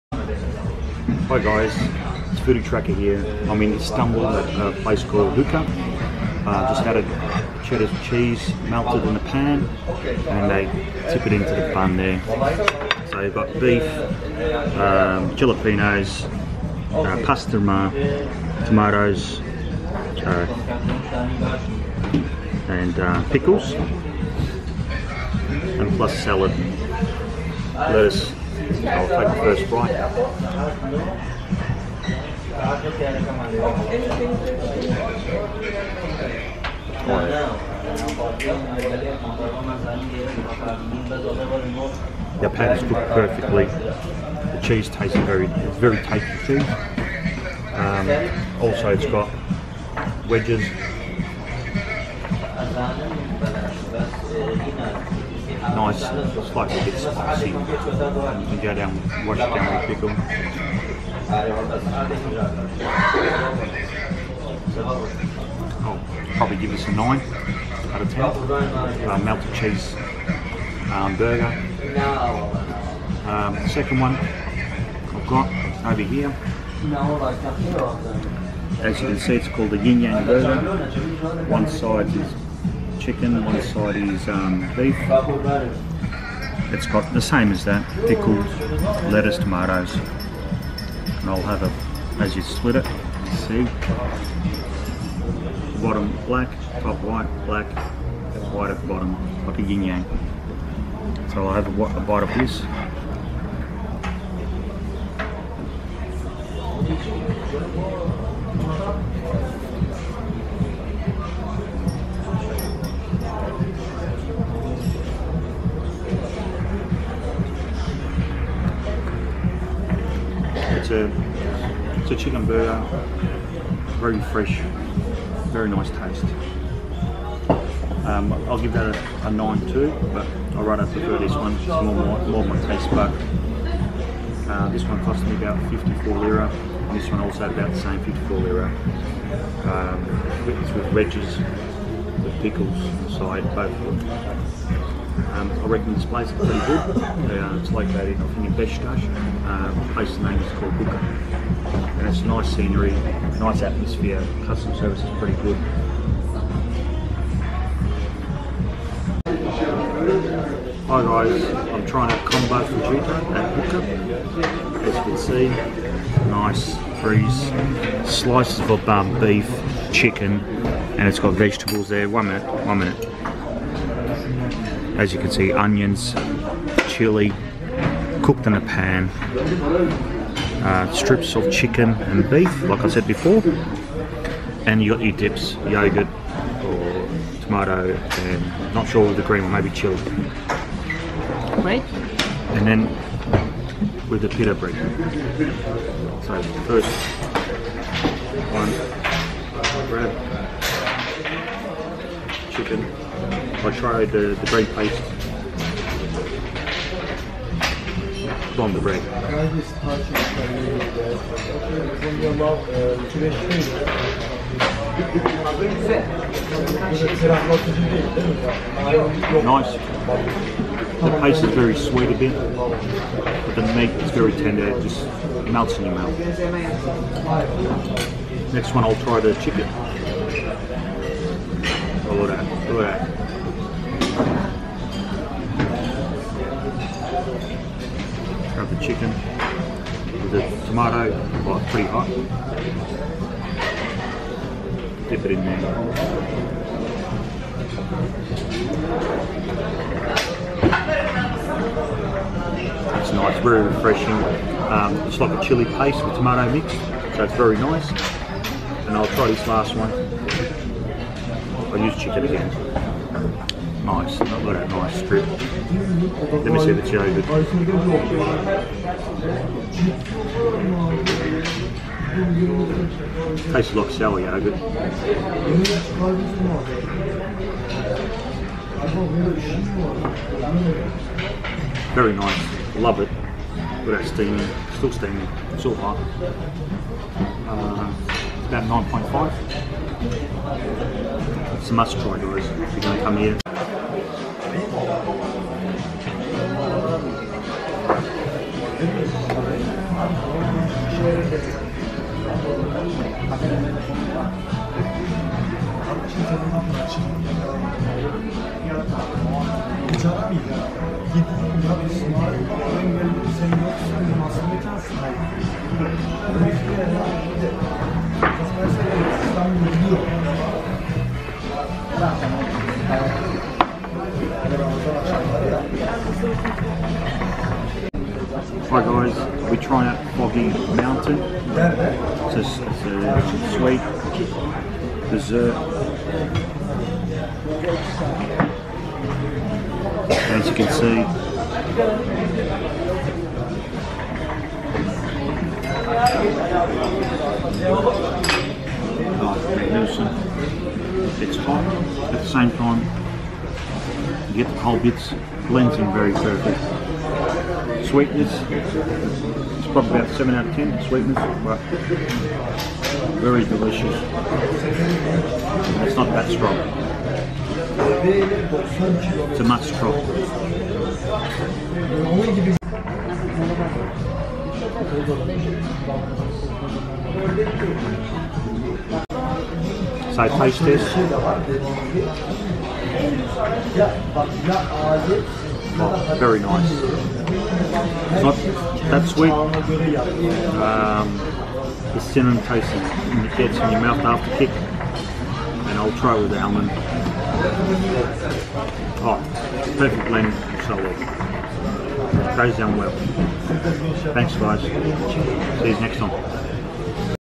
Huqqa Istanbul /Yin & Yang Burger /Melted Cheese Burger / Foggy Mountain dessert /Meat Combo/ Salep
Huqqa Restaurant
 Pizzas, international cuisine & shisha in a modern alcohol-free restaurant with Bosphorus views.
Address: Kuruçeşme, Kuruçeşme Mah, Muallim Naci Cd. No:56, 34345 Beşiktaş, Turkey
Taste tested
Melted Cheese Burger
Yin Yang Burger 1:16
Meat Combo 2:38
Foggy Mountain Dessert  4:53
Salep  6:13